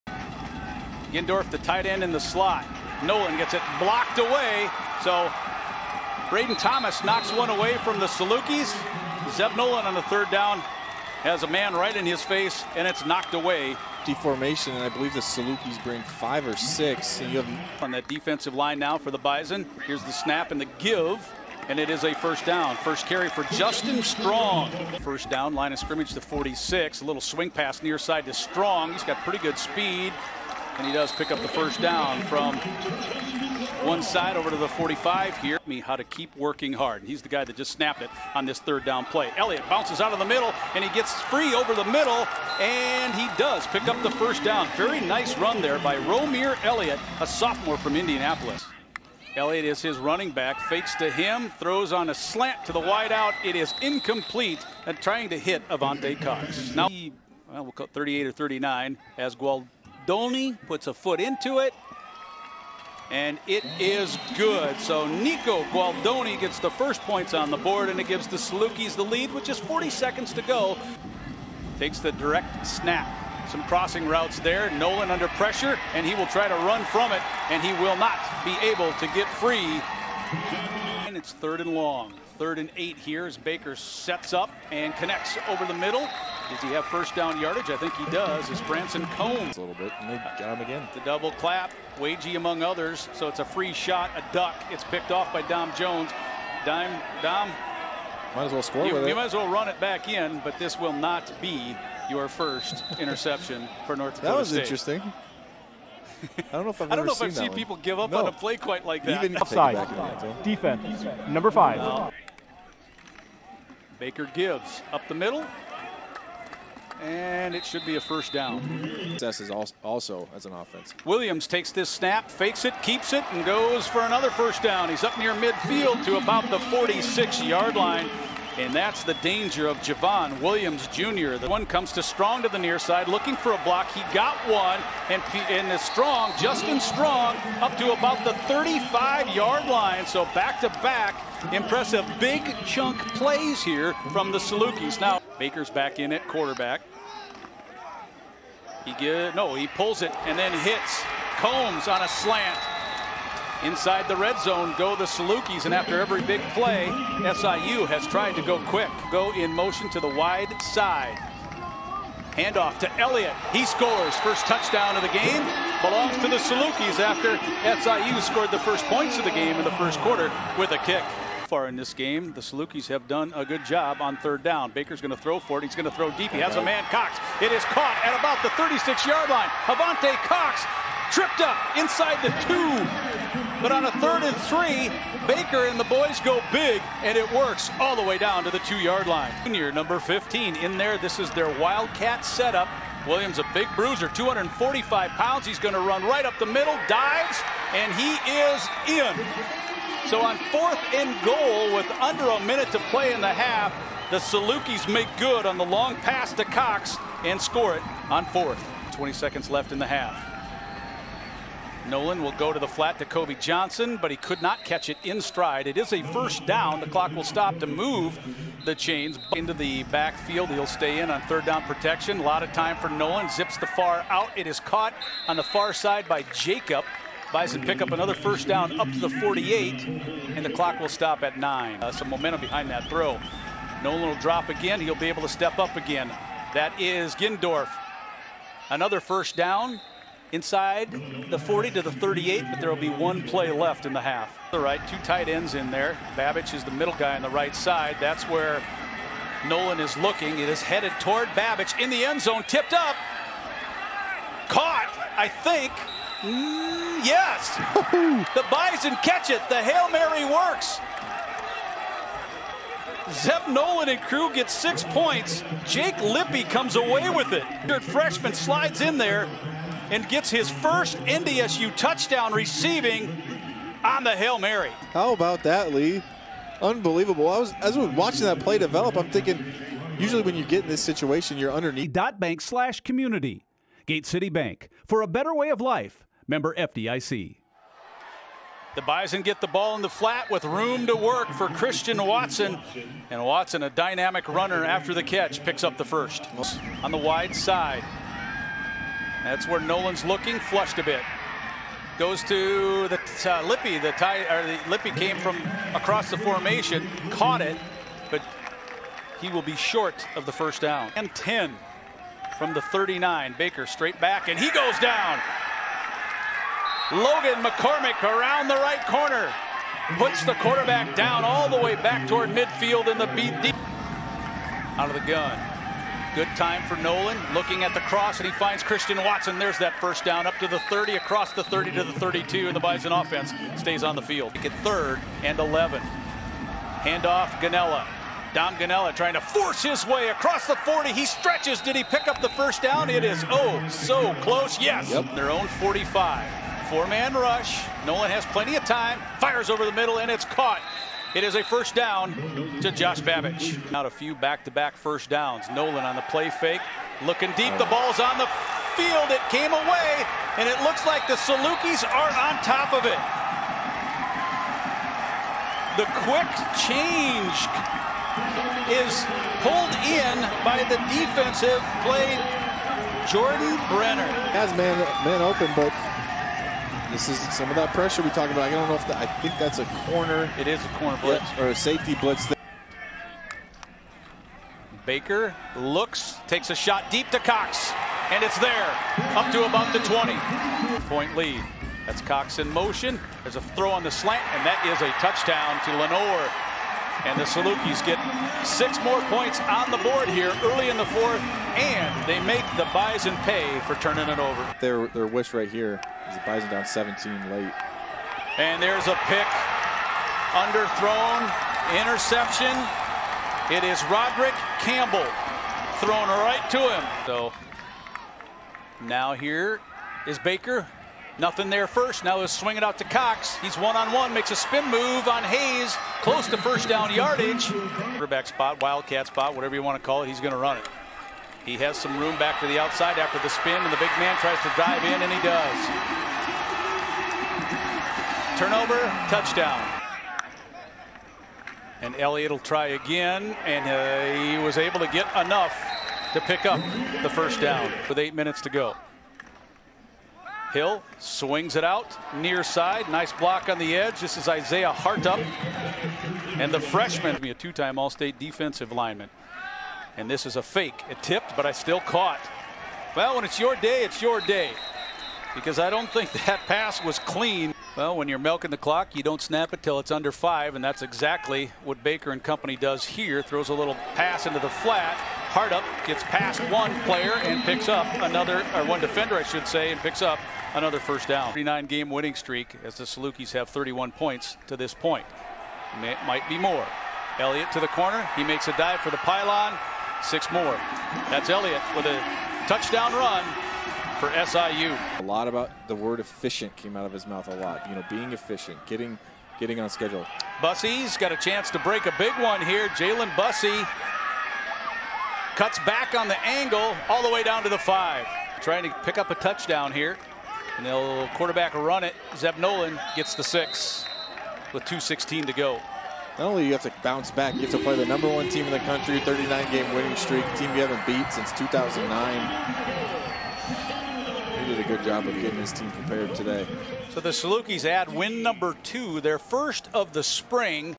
#1 North Dakota State vs Southern Illinois Highlights | 2021 Spring College Football Highlights
North Dakota State vs Southern Illinois Highlights (Southern Illinois vs North Dakota State) College Football Highlights 2.27.21